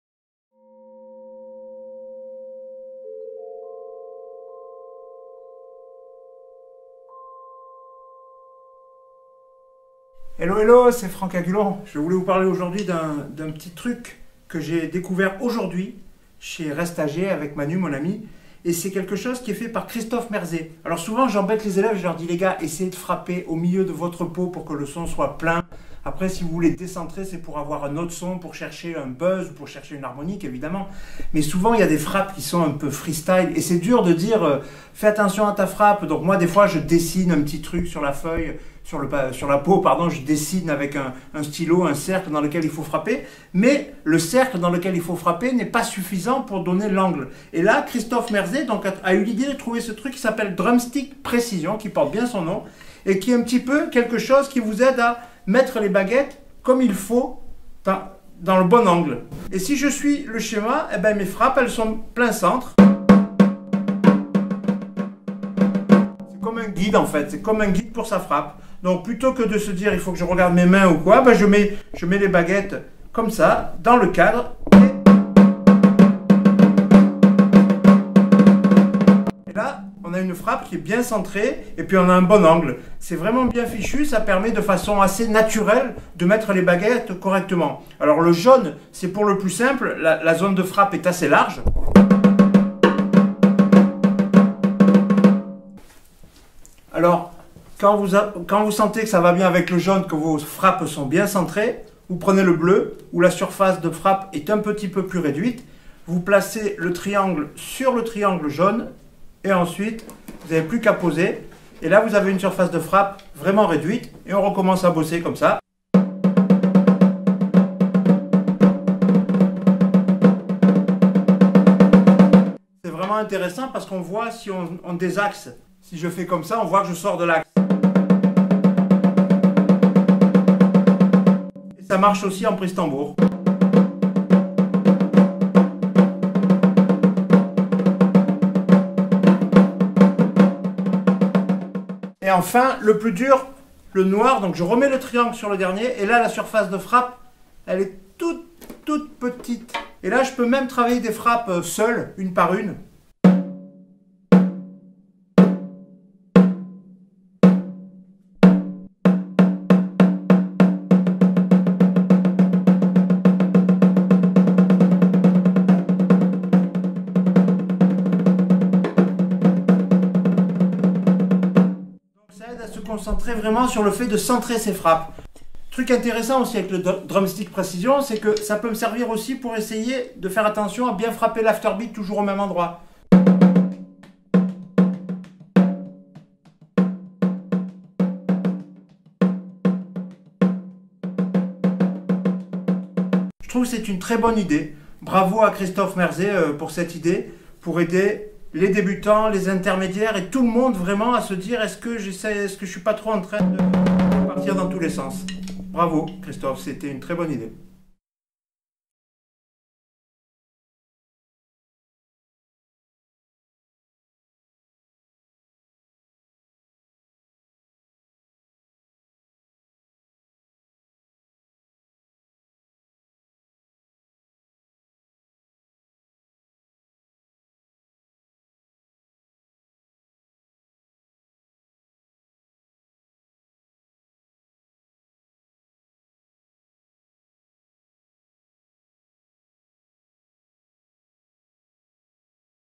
Drum Sticks Precision "DSP" // Le test par Franck Agulhon - Resta-Jay
Franck Agulhon découvre le Drum Sticks Precision, une invention de Christophe Merzet distribuée exclusivement par Resta-Jay percussions.